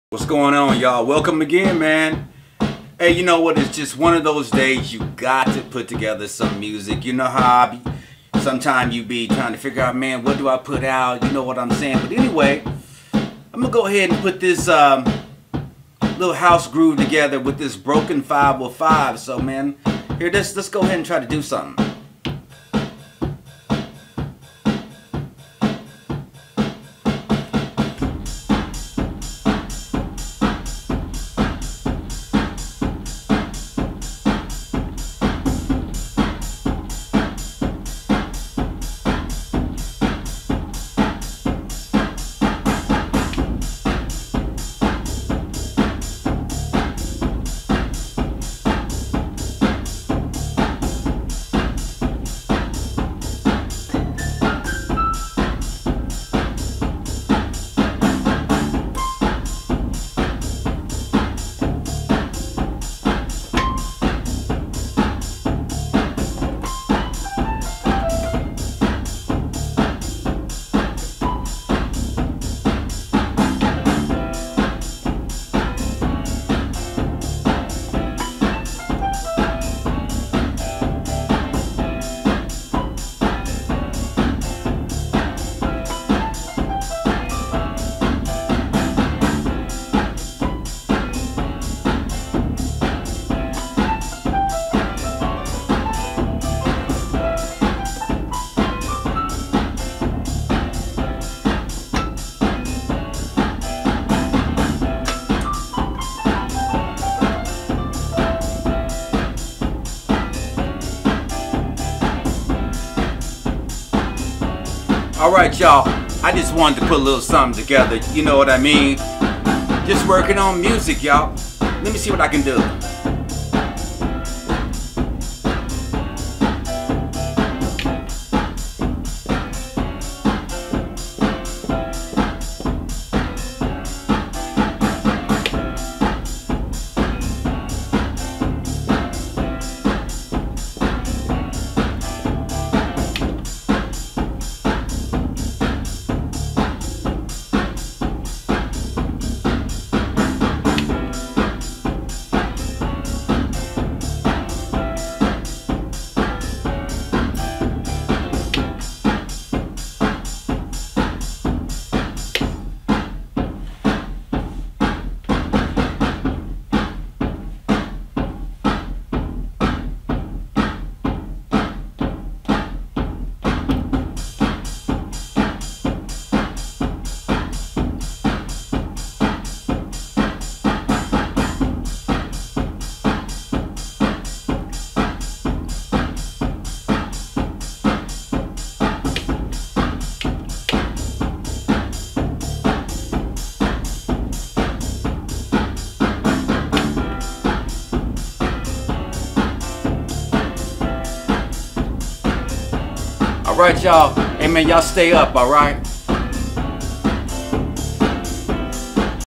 Making Old School House Music
just making an Old School House groove tonight. Everything is on the improvise tip. The instruments is Alesis qs-6 synthesizer,Alesis sr-18,Akai xr-20. Plus a Roland D-20.